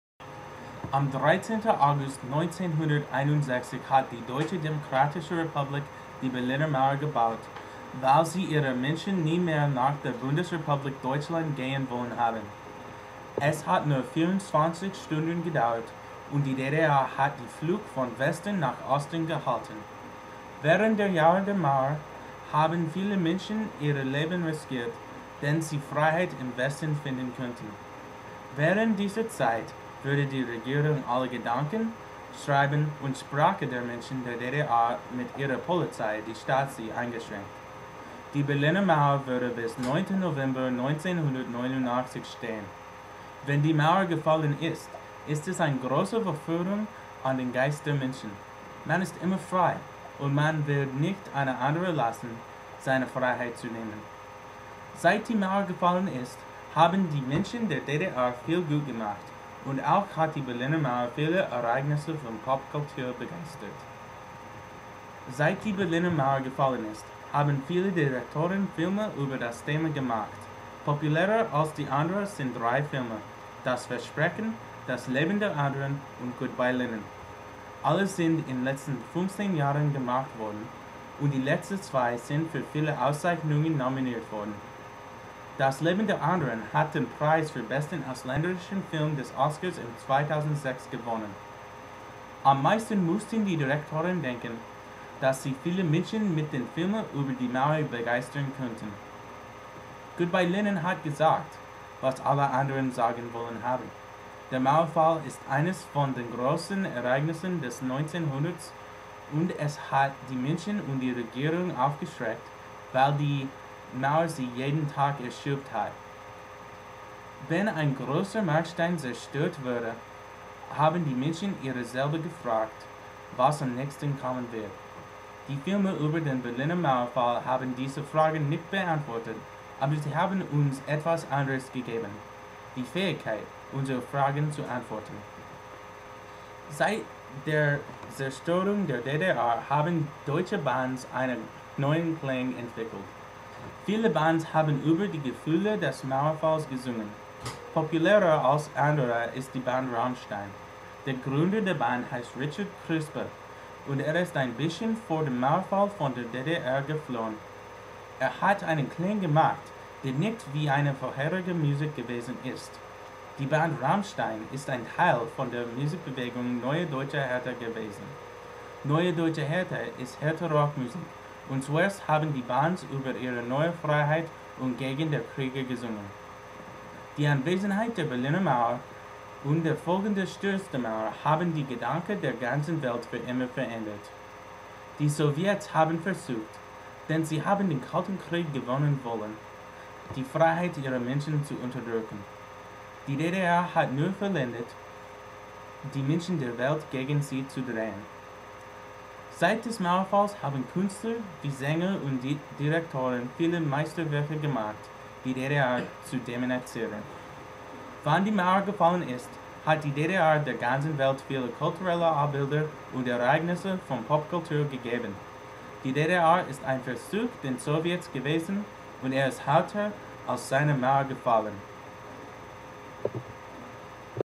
Eine Sprache über dem Berliner Mauerfall
Hier ist meine Sprache, die Affekte des Mauerfalls zu erforschen. Ich hoffe, dass meine Deutsch sehr richtig ist, denn ich eine gute Note in meiner deutschen Klasse bekommen werde. Ich habe viele auf dieser gearbeitet. Ich hoffe, dass Sie diese Sprache genießen werden.